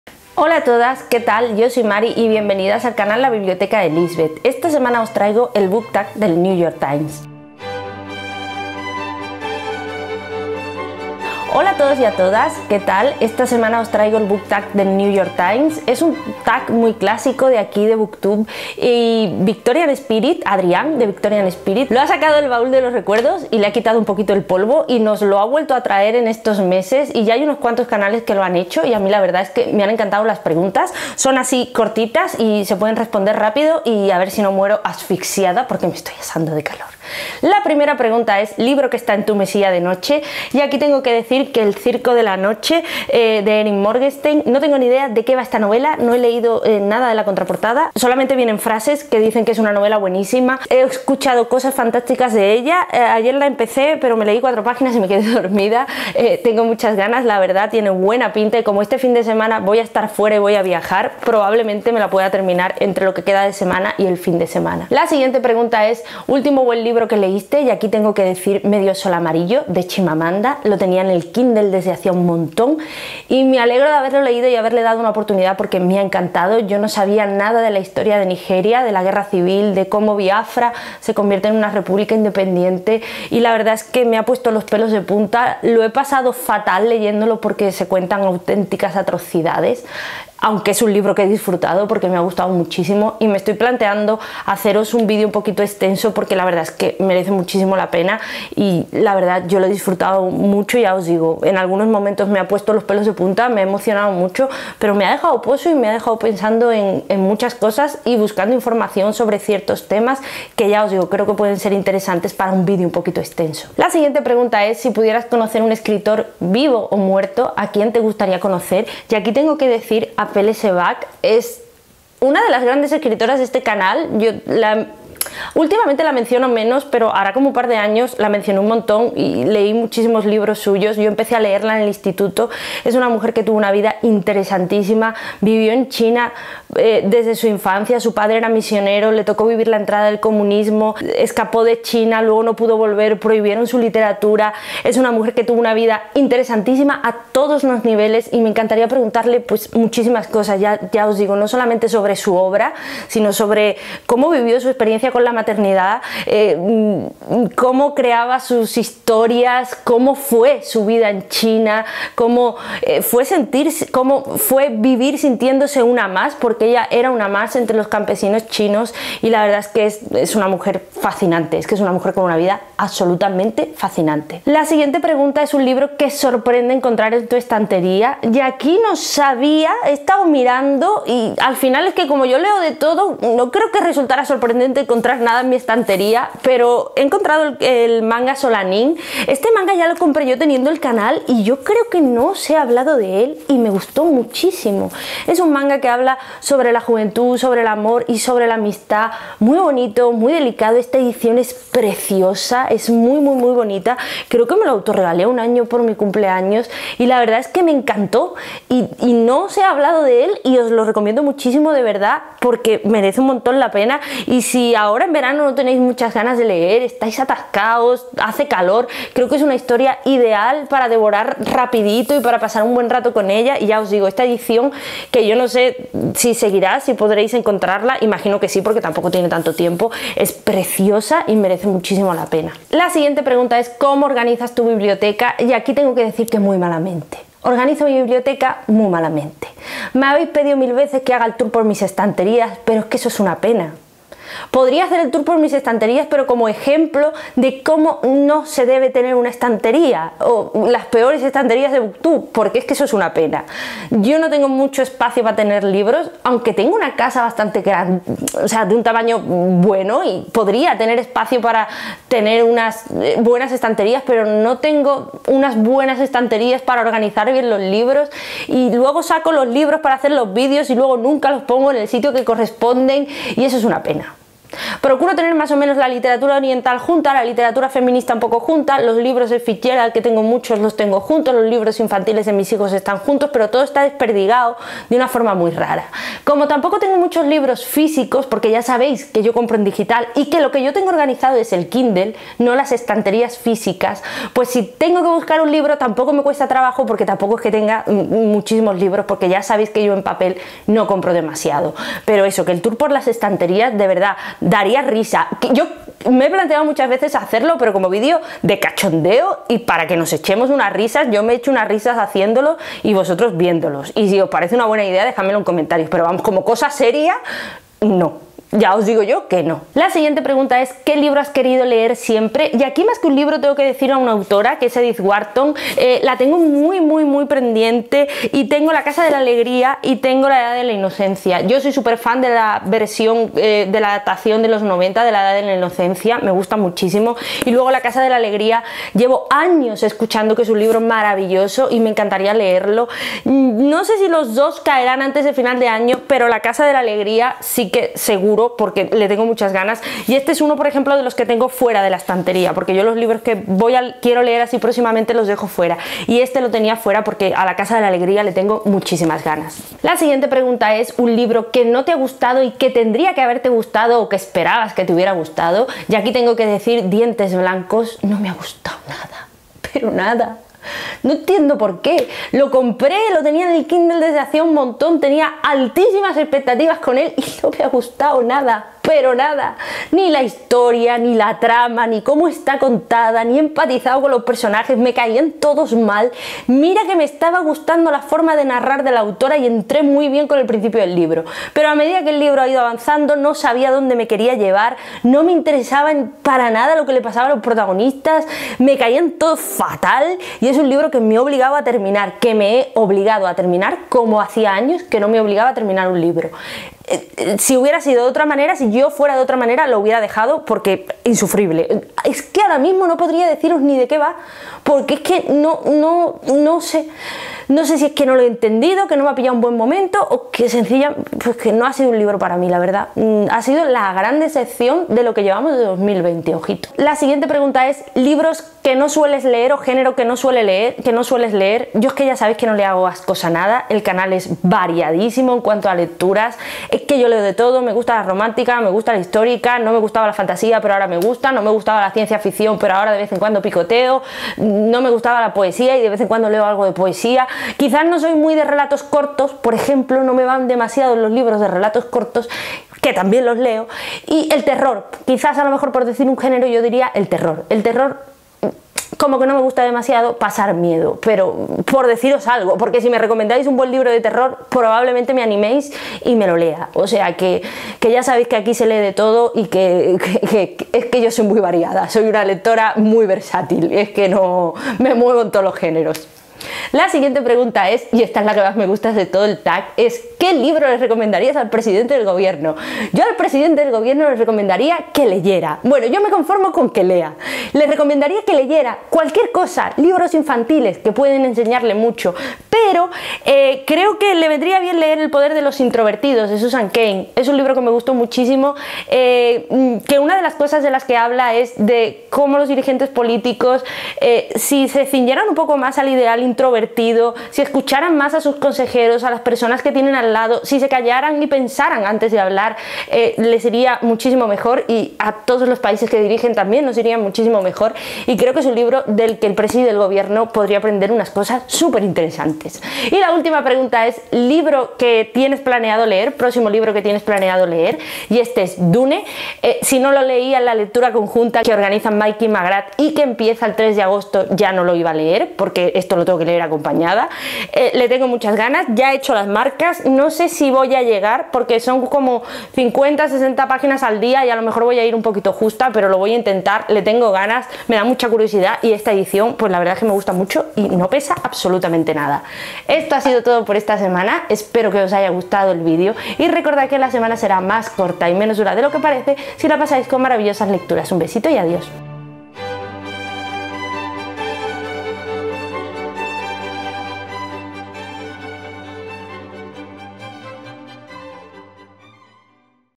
Book Tag The New York Times
Hola amigas y amigos lectores! Esta semana os traigo un vídeo con el Book Tag  The New York Times, todo un clásico de Booktube que estos días ha rescatado el maravilloso Adrian, del que os dejo su canal.